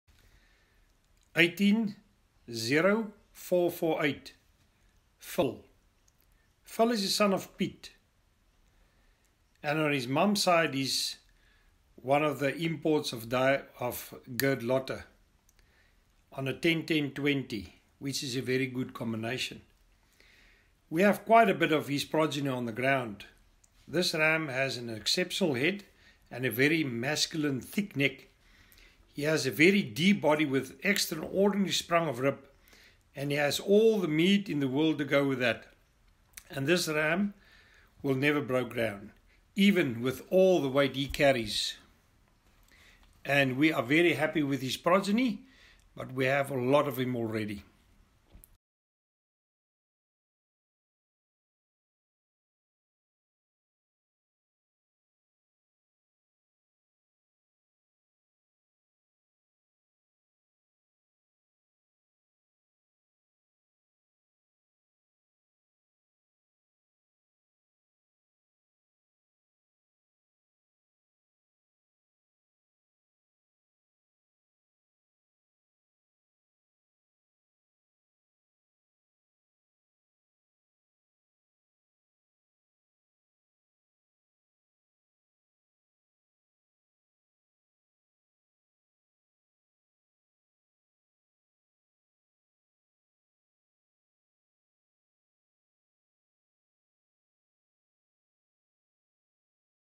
LOT 18 'PHILL' TAG 180448
PHILL
TAG 180448
PHILL is a PIET son and, on his mum’s side, he is a direct import on a Gert Lotter on a 101020, which is a good combination.

We have quite a bit of his progeny on the ground. This ram has an exceptional head and a very masculine thick neck. He has a very deep body with an extraordinary sprung of rib. He has all the meat in the world to go with that, and this is a ram that will never break down, even with all the weight that he carries.